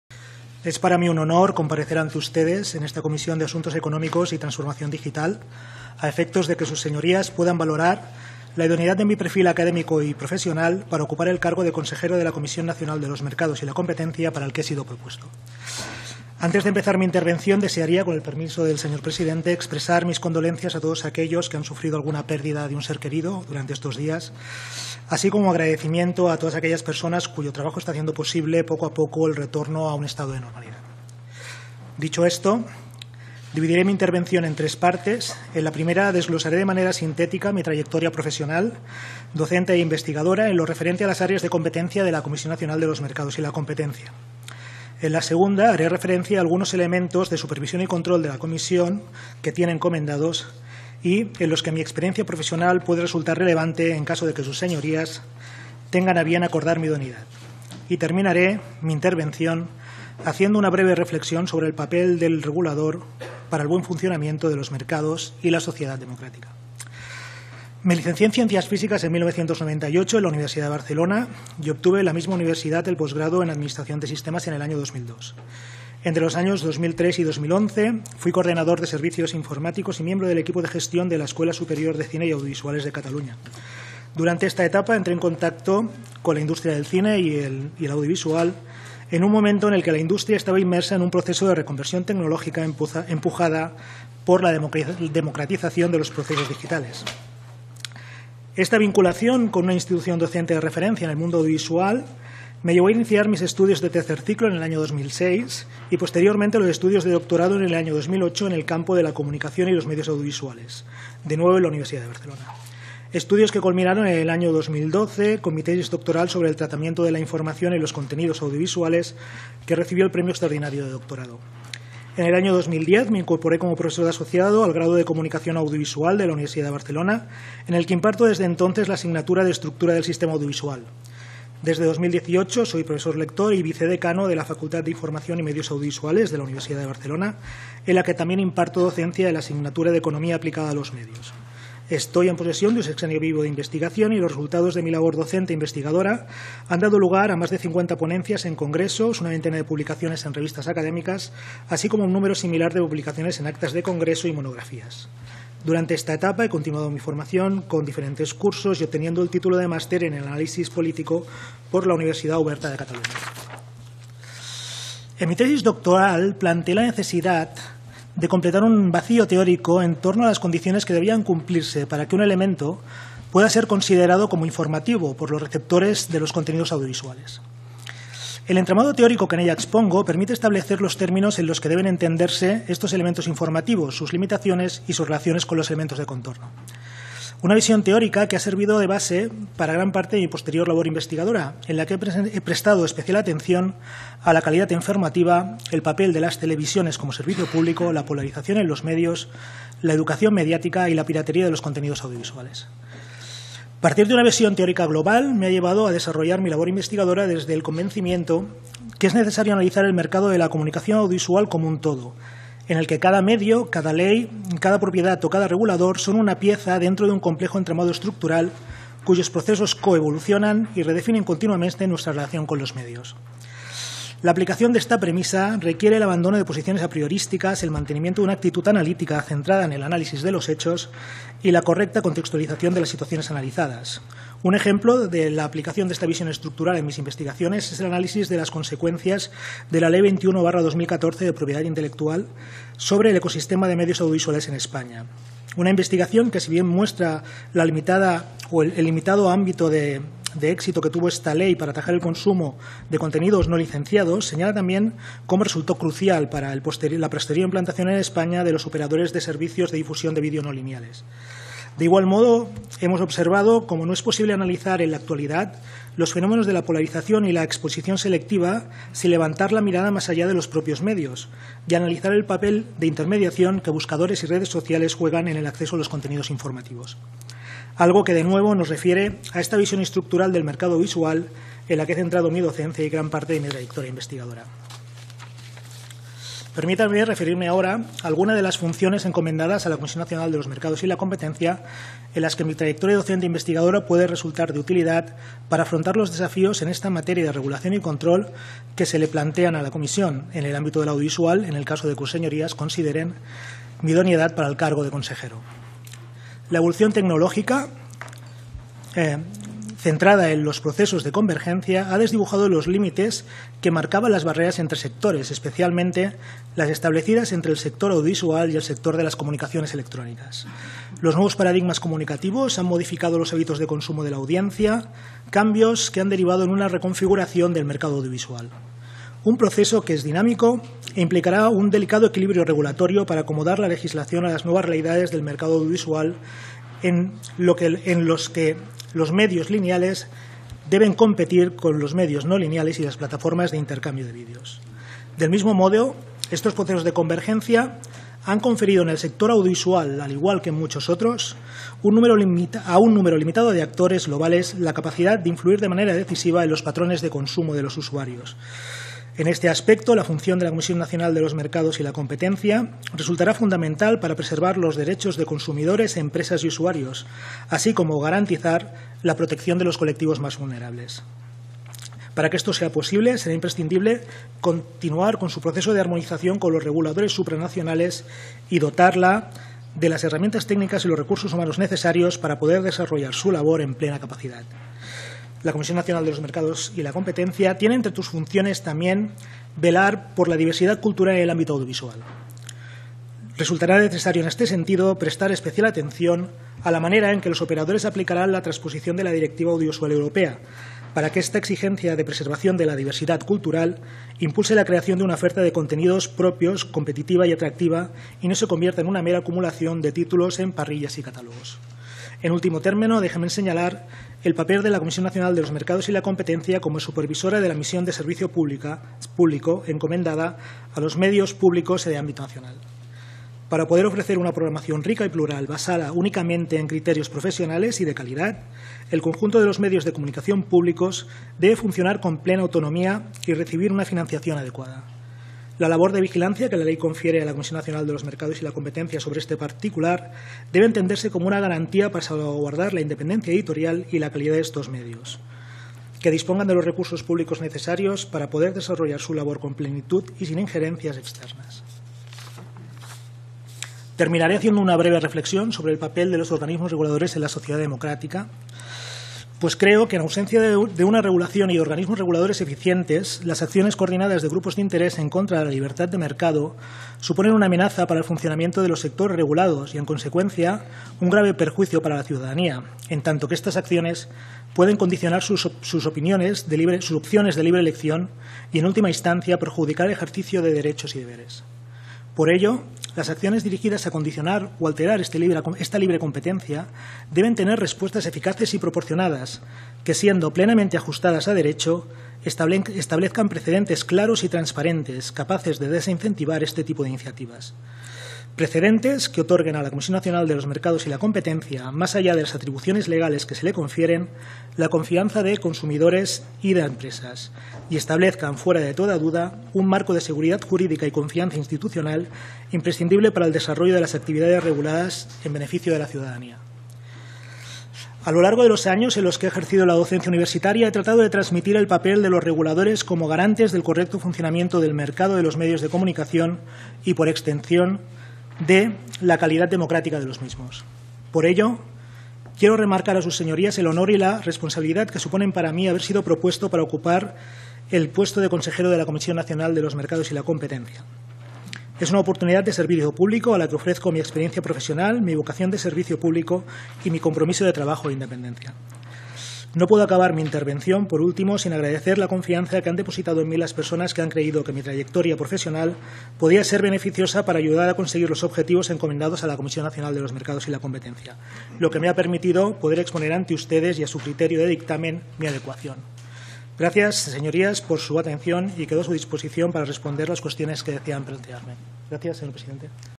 Intervención de Carlos Aguilar Candidato del Gobierno para Consejero de la CNMC. 11 de Junio 2020
Intervención de Carlos Aguilar Candidato del Gobierno para Consejero de la CNMC, en la Comisión de Asuntos Económicos y Transformación Digital el día 11 de Junio de 2020 Recuperado de